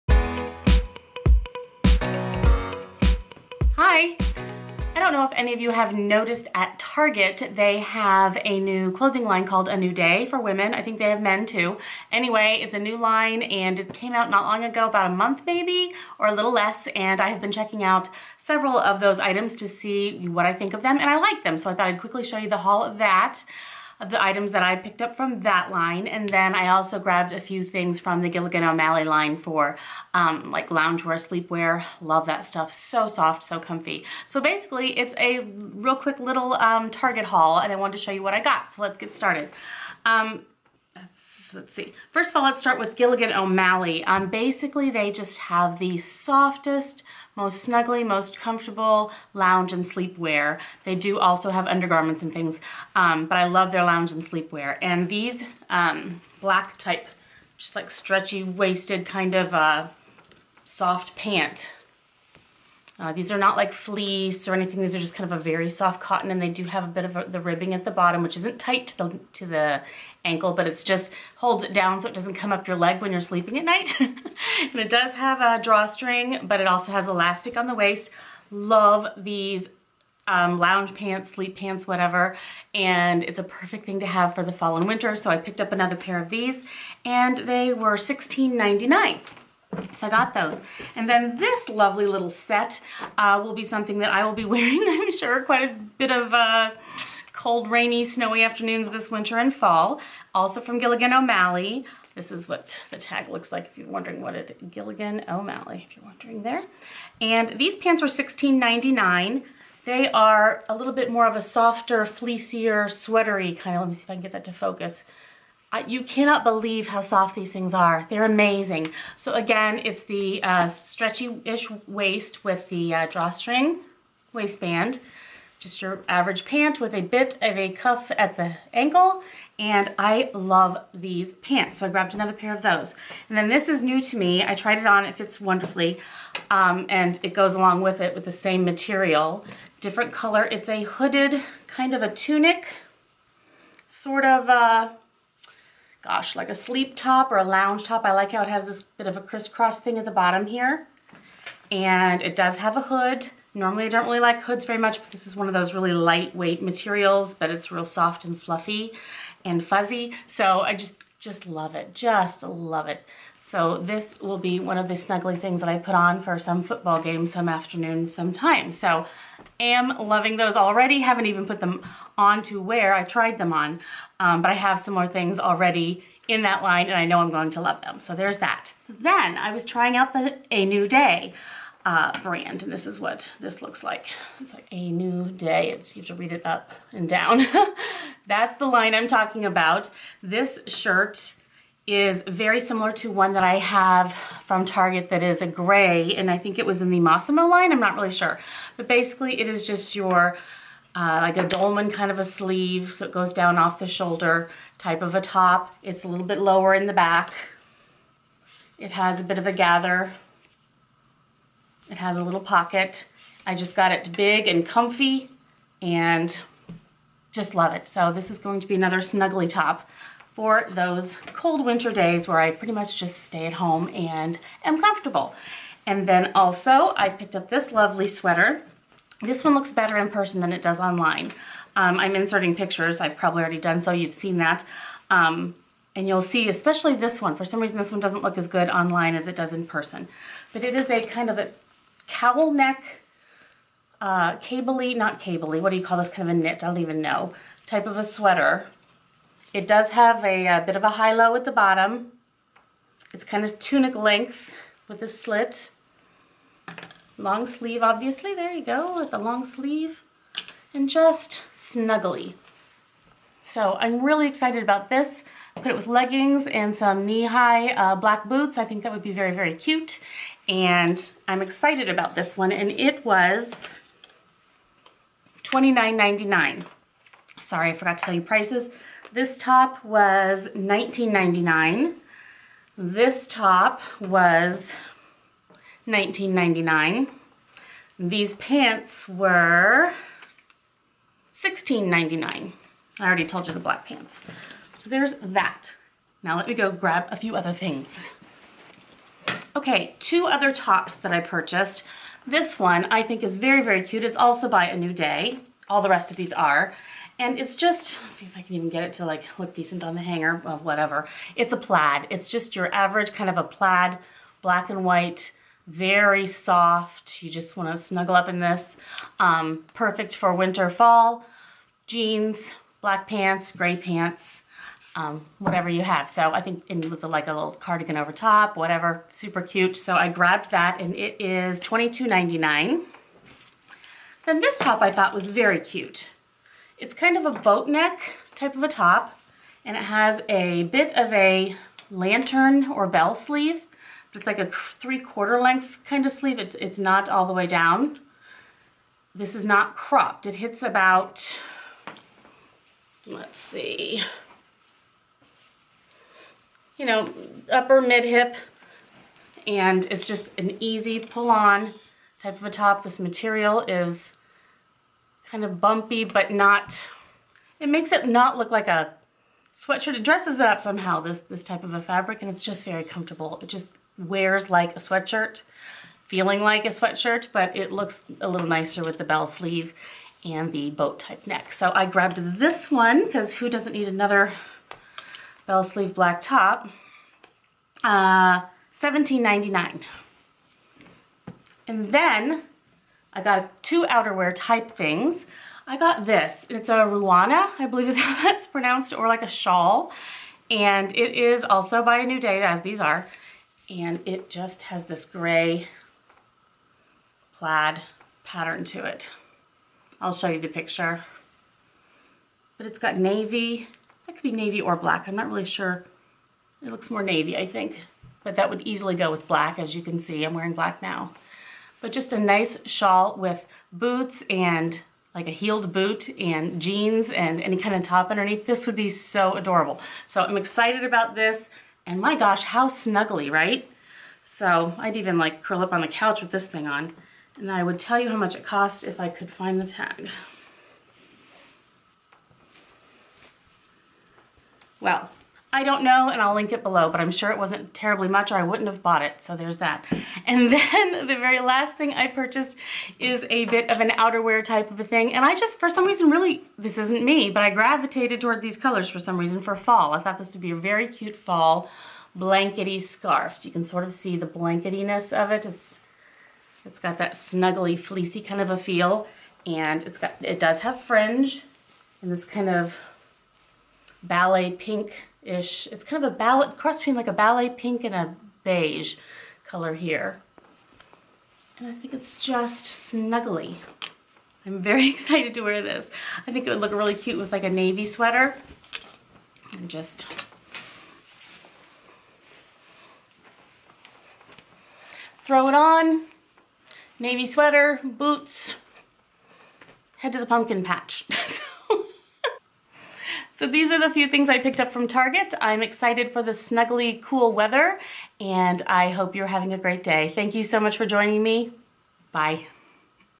TARGET Fall Clothing HAUL
I grabbed a few items from Target recently to add to my Autumn pieces!  Have you checked out their new line...A New Day?

Items Mentioned:
Black Sleep Pants
Grey Lounge Pants
Aqua Lounge Hoodie
A New Day Blue Top
Grey Cowl Neck Sweater
Black and White Plaid Plaid Top
Black Bell Sleeve top (similar)
Plaid scarf

Jennifer V.
U.S.A